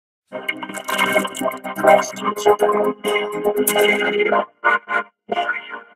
Klasky Csupo in Electronic Sounds {NO AUDACITY OR MAGIX MUSIC MAKER}
I Finally Found The Mda Vocoder And Now I Can Vocode Things Without Audacity And Magix Music Maker!